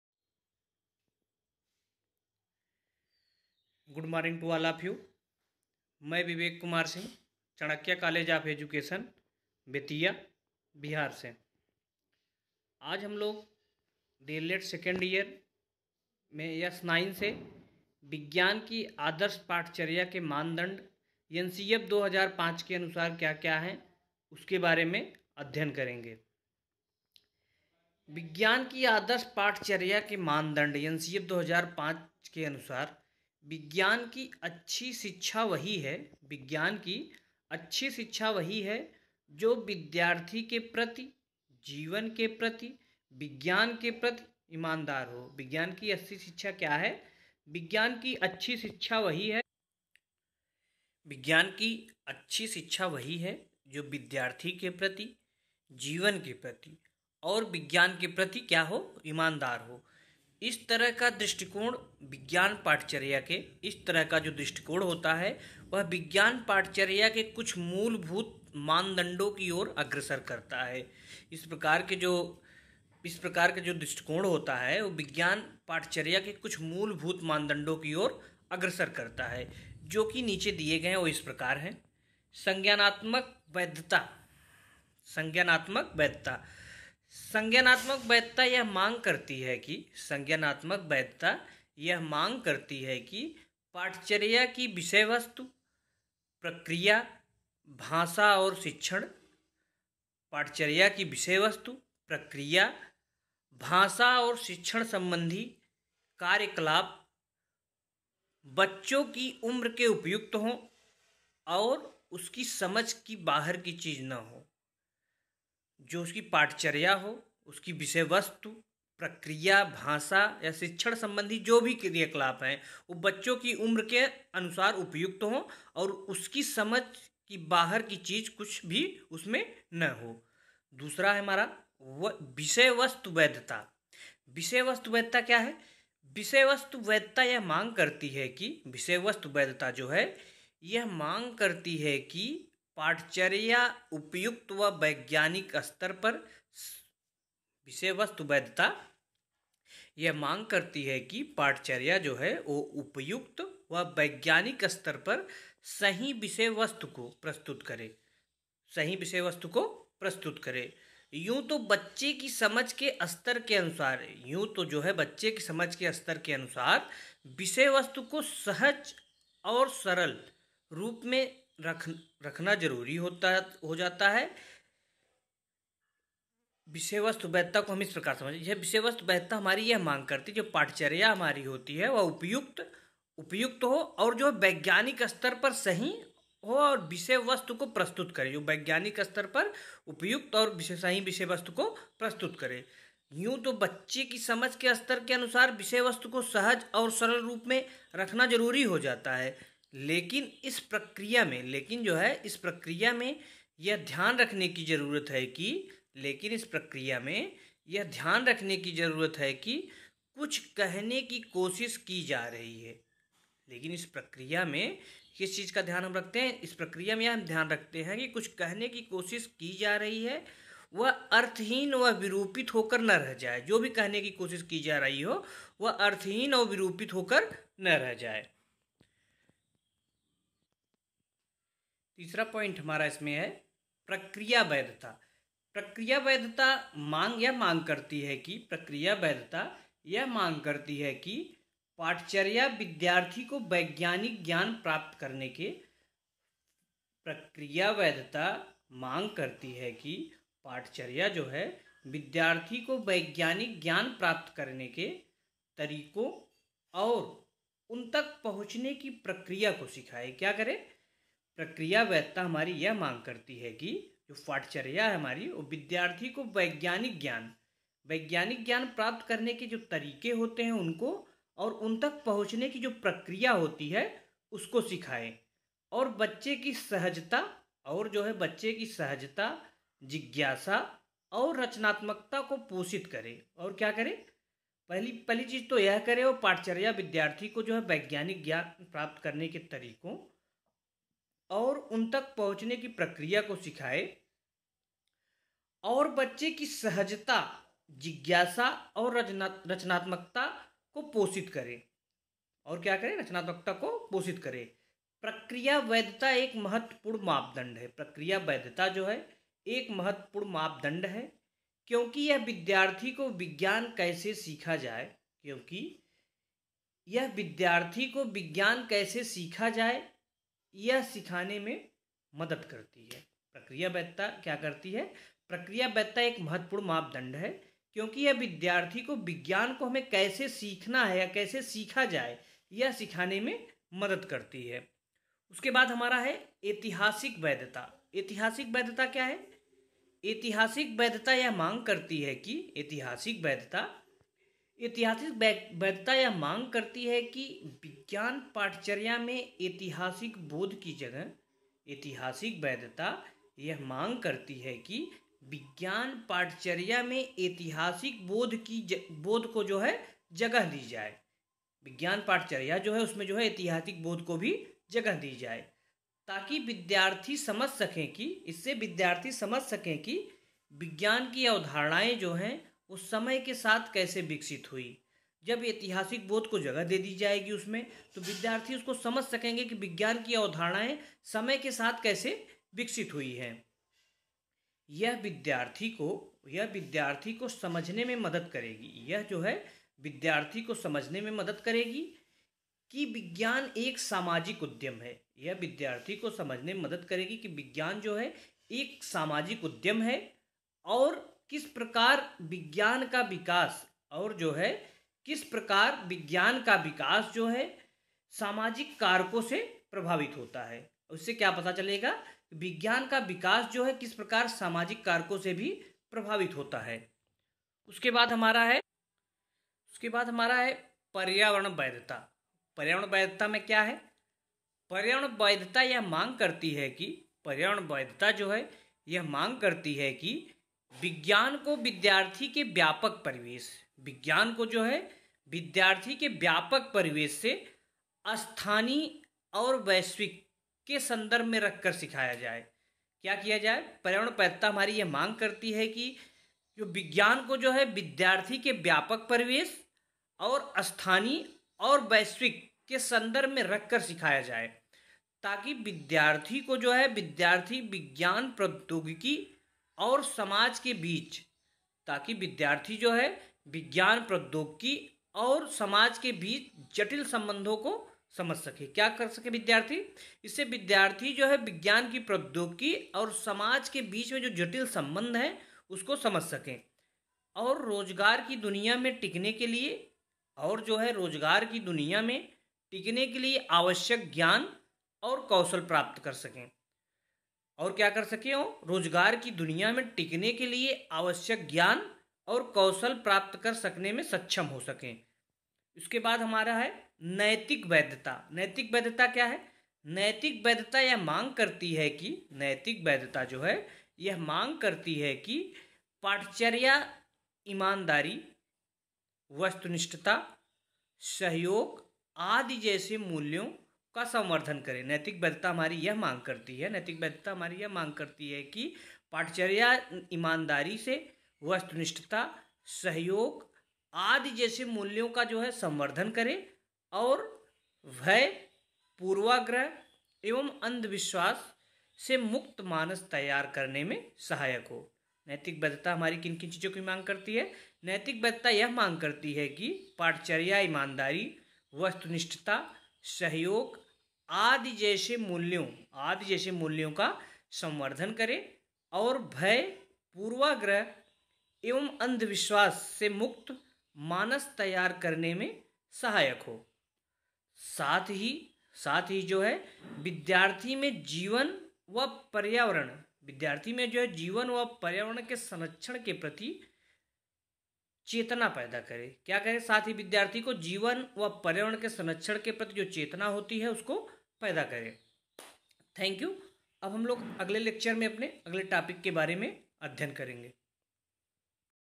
Lecture - , S-9, Unit 1, Topic - NCF 2005 के अनुसार विज्ञान पाठ्यचर्या के मानदंड
Prof. Vivek Singh